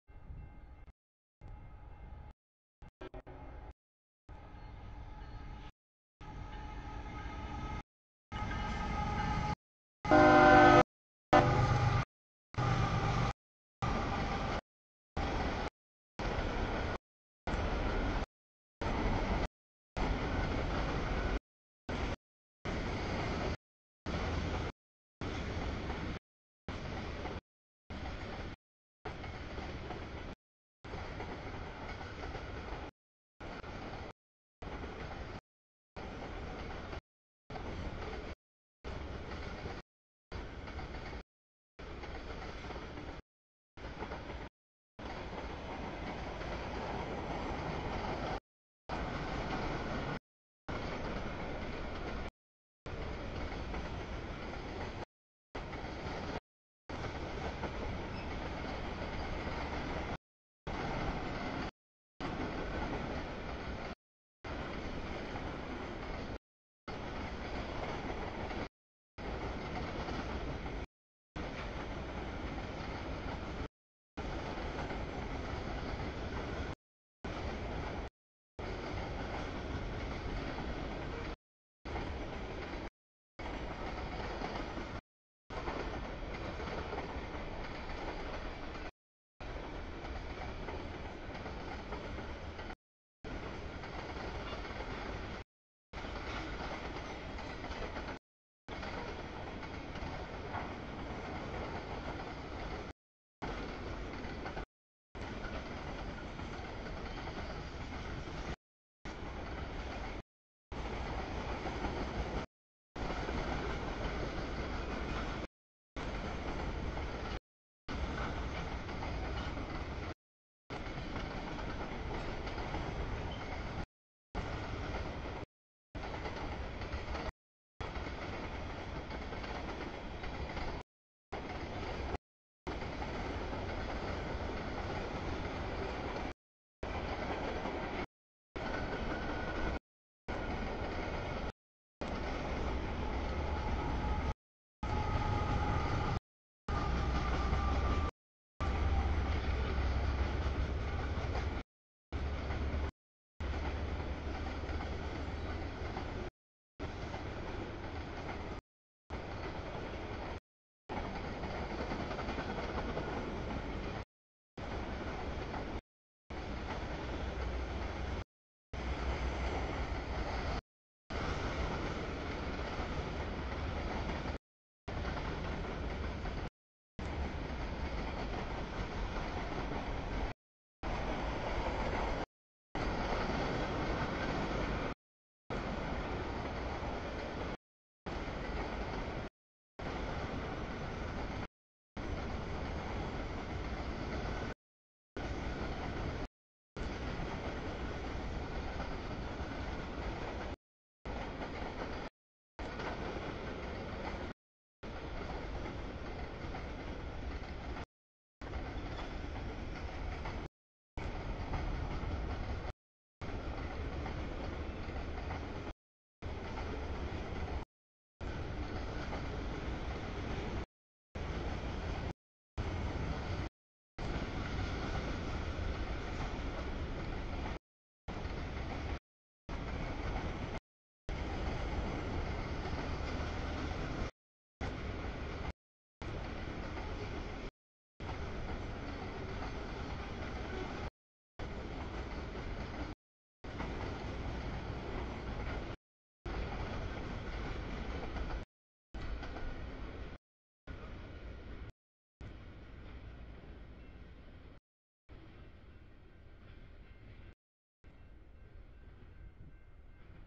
3- 16-2019 05:09 sb mix CSX 3261 Q403
00:01 sb stack CSX 823 Q162
00:19 nb stack CSX Q157
00:37 nb stack trailers CSX Q009
02:20 nb stack CSX Q159
03:21 nb auto rack CSX Q261
05:09 sb mix CSX 3261 Q403
05:18 nb empty's triple BNSF K143
06:06 sb stack CSX DPU Q160
06:26 sb mix CSX Q409
06:46 sb stack CSX Q008
06:51 nb stack trailers CSX 5375, 743 Q003
07:18 nb stack trailers CSX 3276, 389 Q007
07:59 sb stack trailers CSX 3428, DPU 3417 Q010
09:45 nb auto rack CSX 531, 78 Q253
09:54 nb empty's BNSF 4154, 8238 CEFX 1041 K163
10:45 sb stack CSX 5413, 3038 Q158
11:14 sb auto rack CSX 5267, 7347 Q234
11:35 nb mix BNSF 6856, 5760 CSX 3121, 211 Q190
12:20 nb mix, epic/blue trash CSX 14, 5438 Q434
13:55 nb nysw cut, auto racks, epic trash CSX 3067, 921, 3031 Q410
15:50 sb tanks CSX 8009 BNSF BNSF K160
16:31 sb green trash, mix CSX 824 DPU 949 Q439
17:27 sb tanks CP 9774, 8304 K602
17:34 nb empty's UP 7913 KCS 4748 K649
18:43 sb mix, blue trash CSX 252, 508 Q433